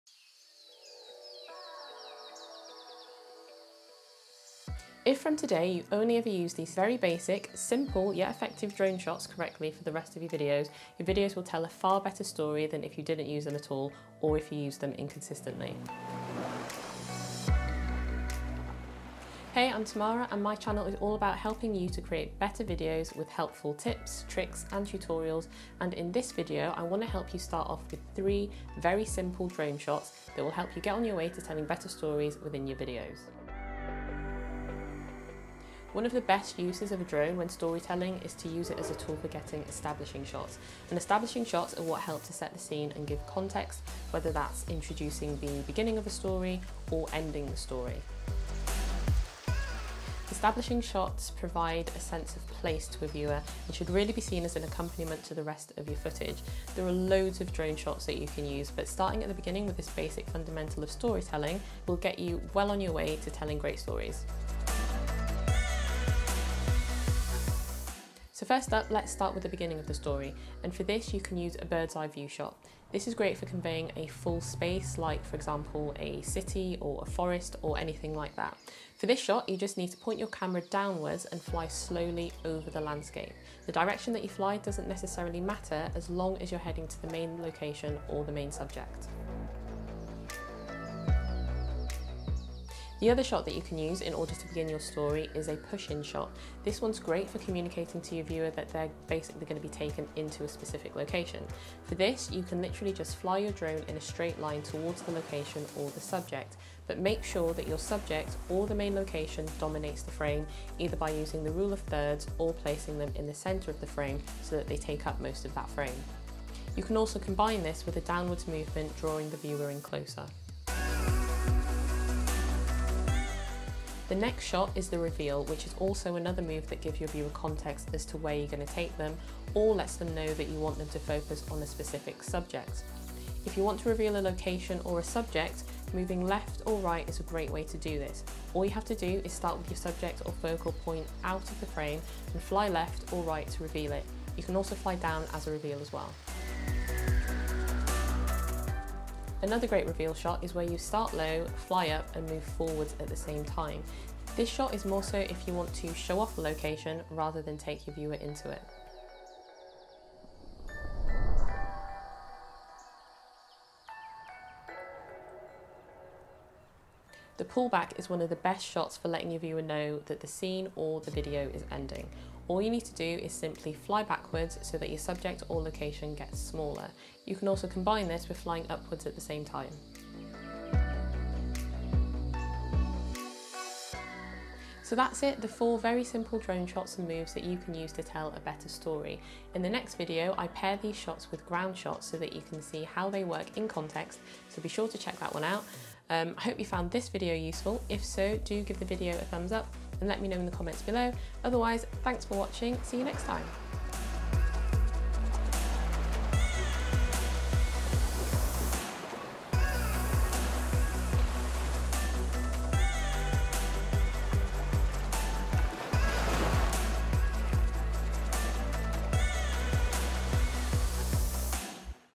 DJI Mini 2 Cinematic Tips // 4 Simple Drone Shots for Basic Storytelling
In this video, I'm going to share some DJI Mini cinematic tips which are my top 4 simple drone shots that you can use as establishing shots for basic storytelling in your videos. Your drone is one the best tools for helping to communicate a better story to your viewer to provide them with context, and in this video, these 4 simple drone shots are ones that you can use right away and put them into practice in your next video and help you think about your drone shots in a bit more context to tell a better story.

The footage in this video was shot with the DJI Mini 2

*** VIDEO CHAPTERS ***
00:00​ - DJI Mini 2 Cinematic Tips
00:35 - Why Use Establishing Shots?
01:05 - Shot 1
01:31 - Shot 2
02:01 - Shot 3
02:45 - Shot 4

Don't forget to say 'Hi' in the comments or let me know if you have any questions.